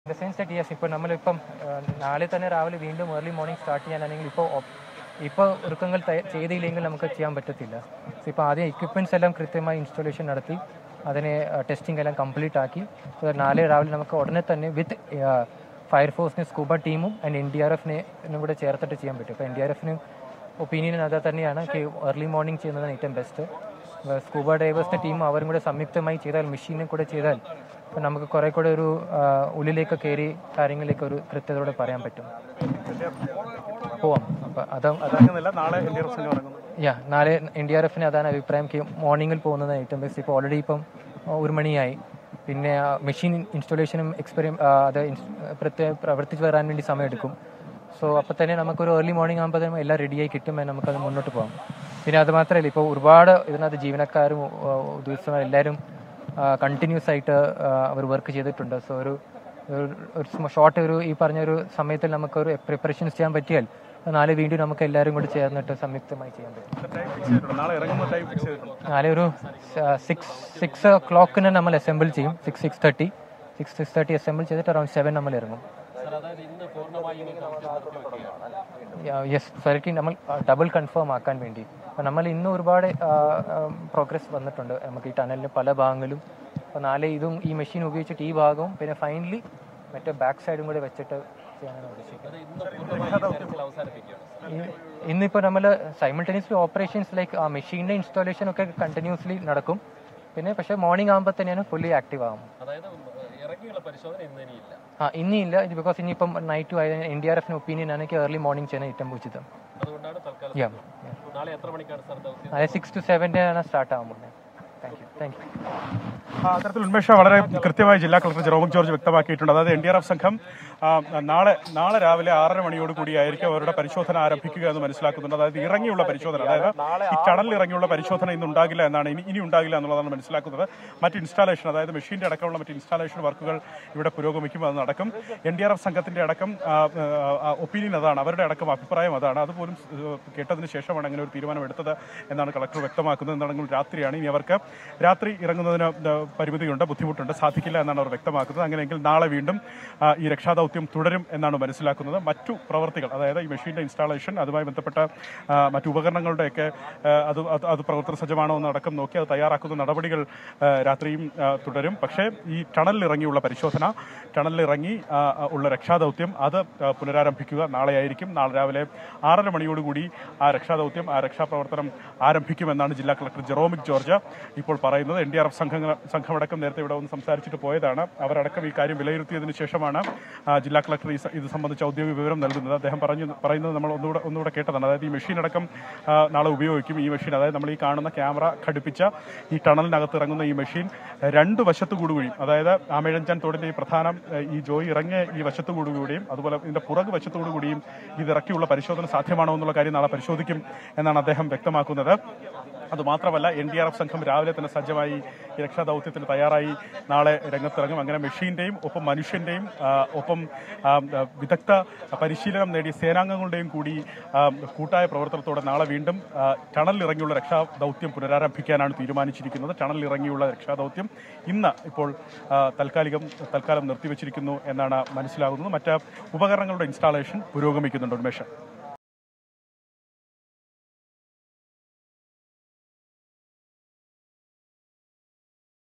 13 മണിക്കൂർ നീണ്ട ഇന്നത്തെ തെരച്ചിൽ അവസാനിപ്പിച്ചു; NDRF സംഘം തെരച്ചിലിനിറങ്ങുക നാളെ
ആമയിഴഞ്ചാൻ അപകടം; 13 മണിക്കൂർ നീണ്ട ഇന്നത്തെ തെരച്ചിൽ അവസാനിപ്പിച്ചു; NDRF സംഘം തെരച്ചിലിനിറങ്ങുക നാളെ

Today's 13-hour search ends; NDRF team will start search tomorrow